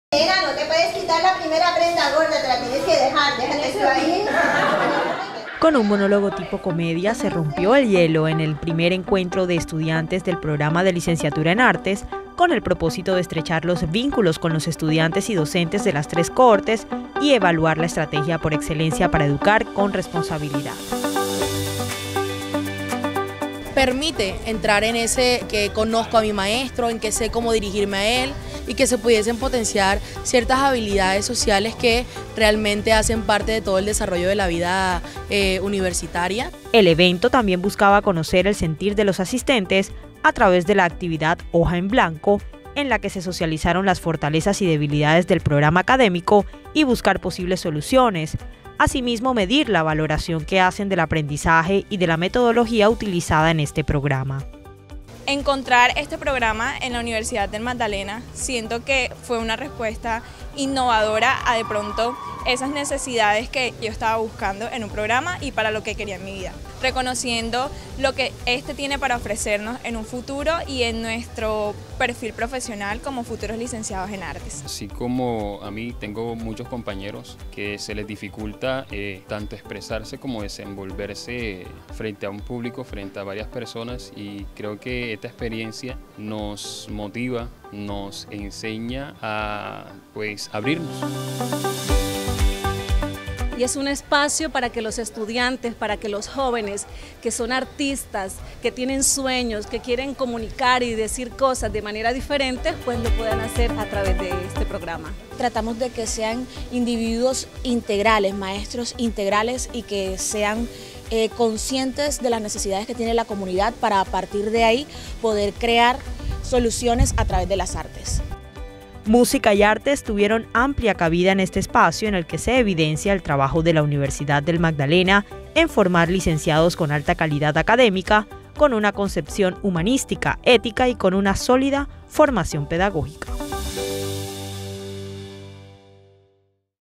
Docentes y estudiantes de Licenciatura en Artes articulan esfuerzos y fortalecen procesos educativos
Para estrechar los vínculos entre sus estudiantes y docentes, el Programa de Licenciatura en Artes realizó el I Encuentro de Estudiantes, en el que el arte y la música fueron protagonistas. El evento también sirvió para conocer inquietudes sobre los procesos educativos y construir estrategias para fortalecer la metodología de enseñanza y aprendizaje individual.